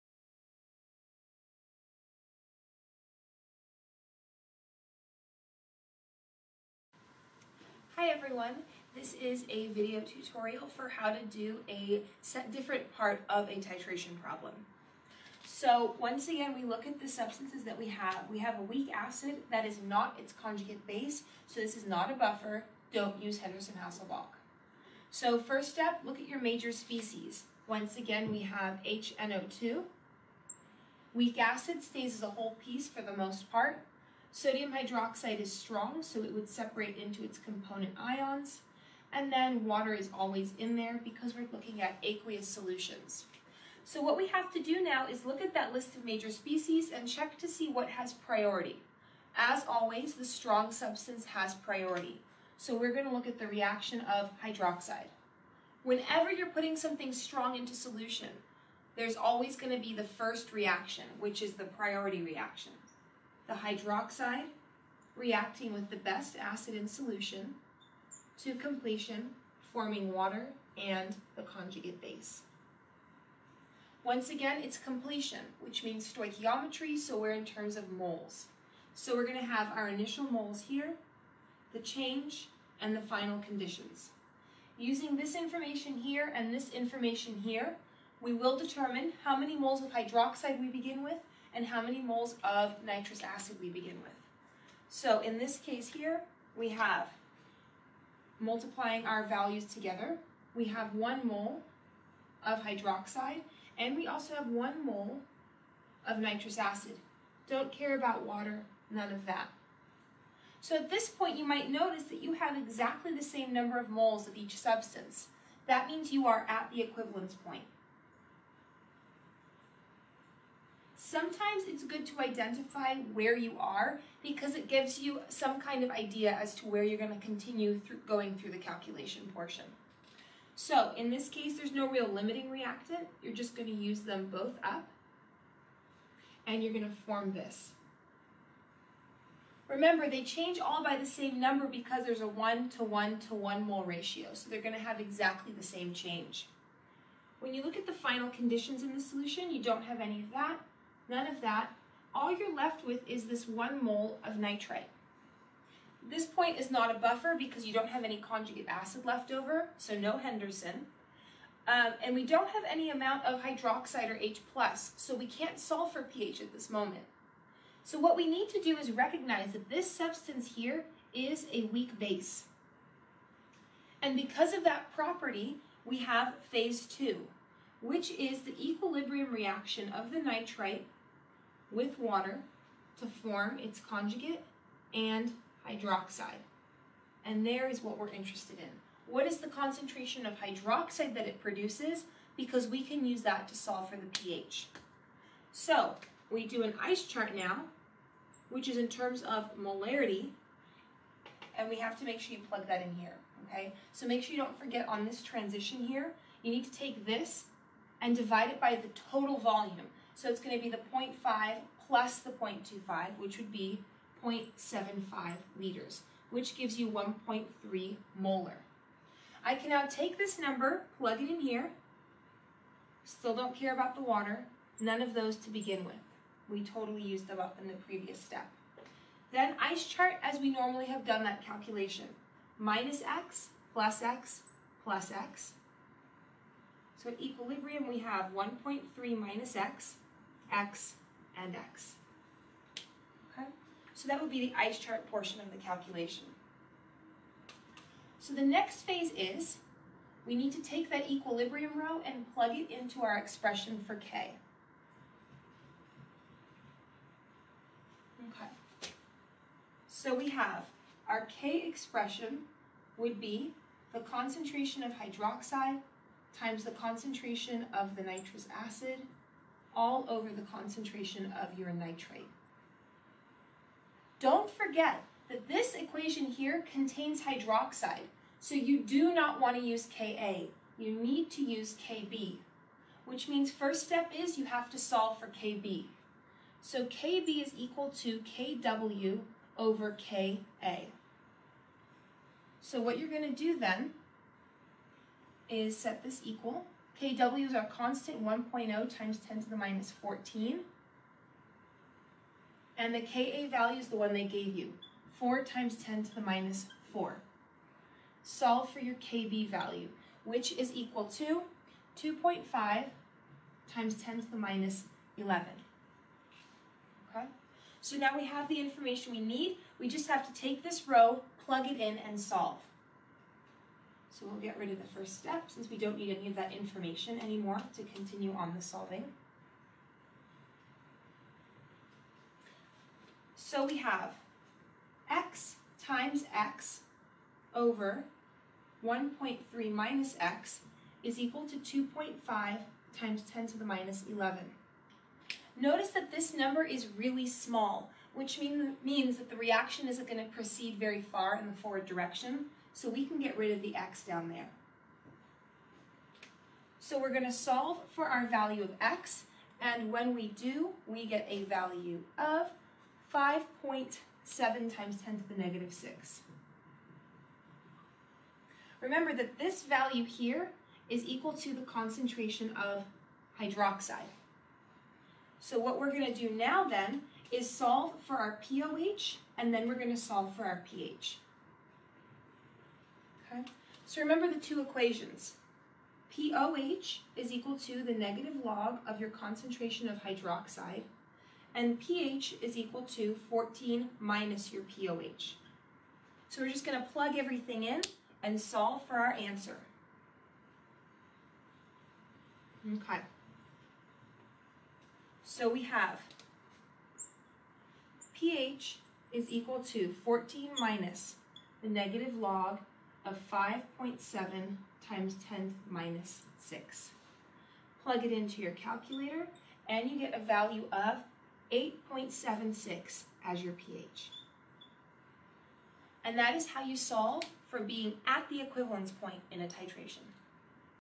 Equivalence Point Titration Reaction Calculation
In this video we look at the method for calculating the pH of a 0.50L solution of 2.0M nitrous acid titrated with 0.25L of a 4.0M NaOH solution.

For more outstanding tutorials and study guides!!!